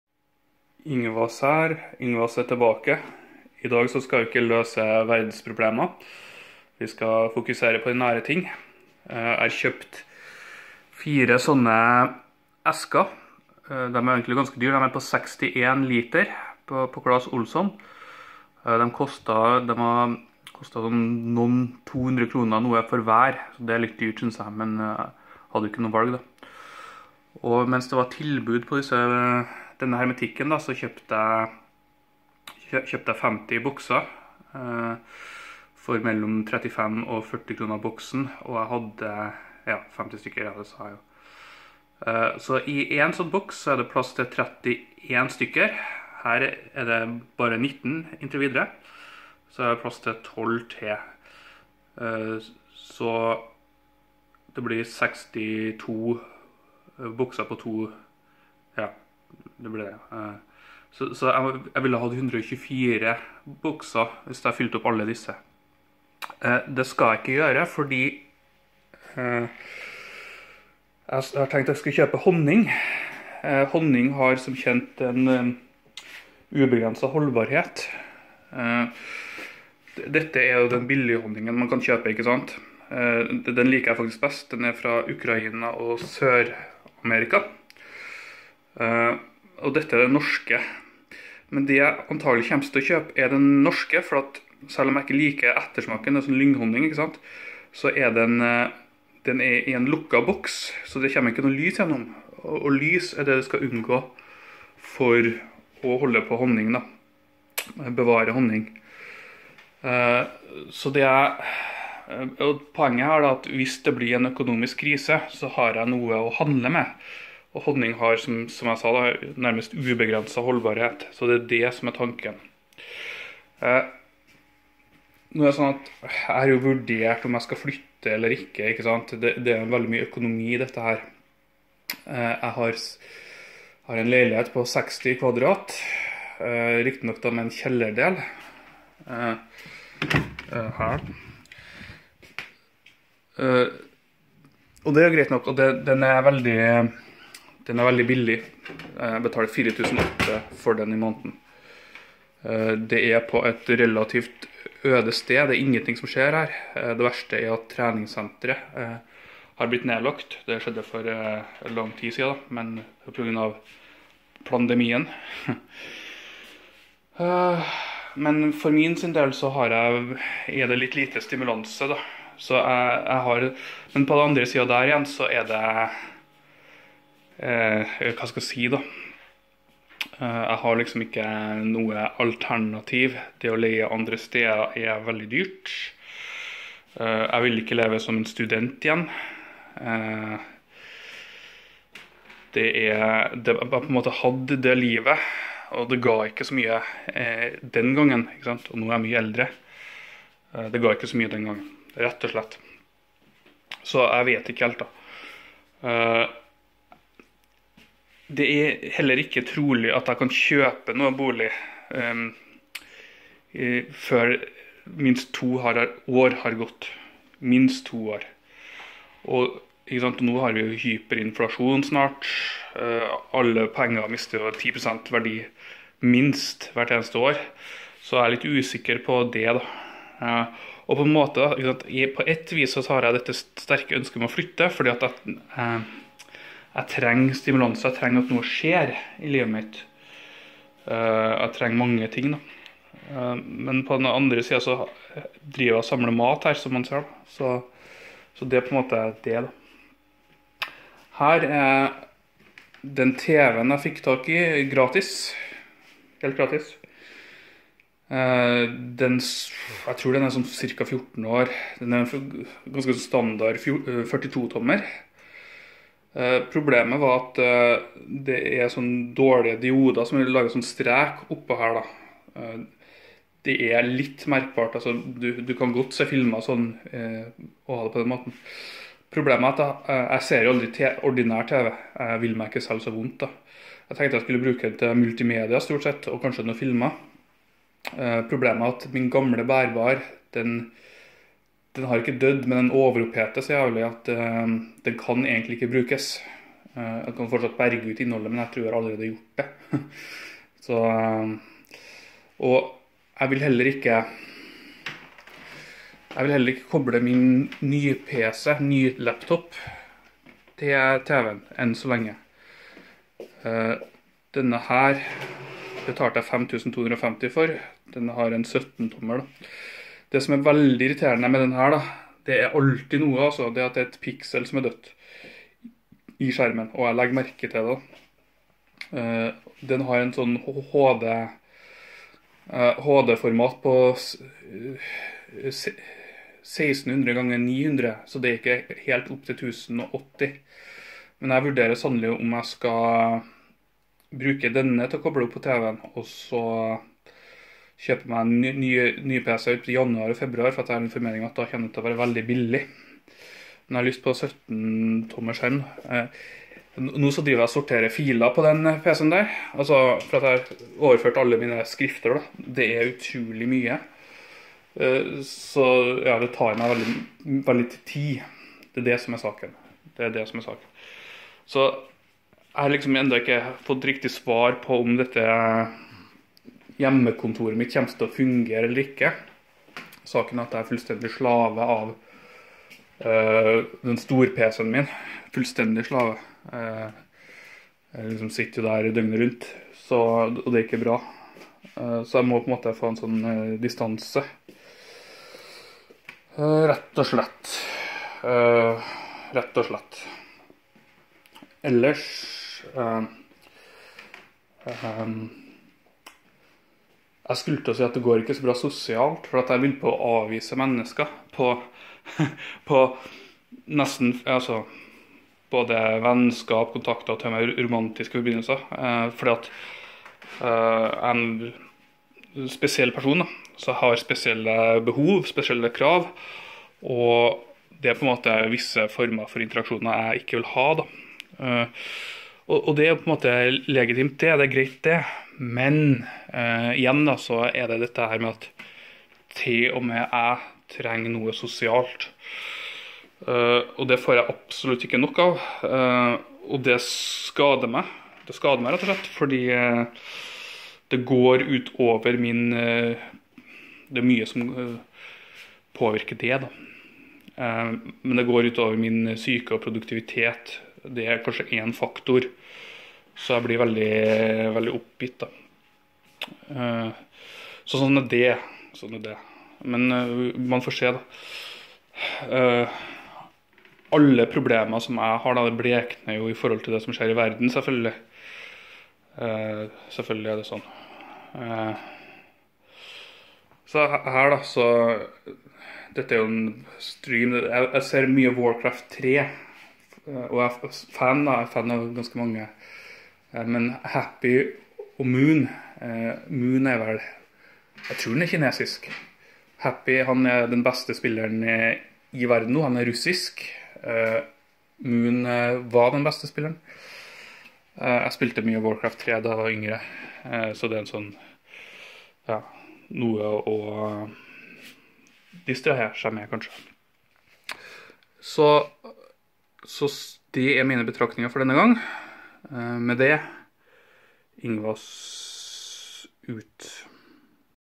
Hermetikk gjemt i gangen
1 hermetikkboks koster gjerne 80 kroner i dag. 10 bokser blir altså 800 kroner. Det er mye penger og setter ting i perspektiv. Jeg kjøpte på tilbud, men det er ikke så sikkert at jeg vil være like heldig neste gang.

Glemte å si at jeg vil legge et stort teppe over disse plastboksene, så det ser penere ut. Kanskje skjærer jeg ut noe papp til å bruke som bordplate øverst.

Hvis jeg kunne velge hadde jeg selvsagt gått til innkjøp av tusenvis av hermetikkbokser, og hundrevis av liter med honning. Per dags dato har jeg ikke lagringskapasitet, og innen jeg skaffer meg en eiendom kan tiden være forbi.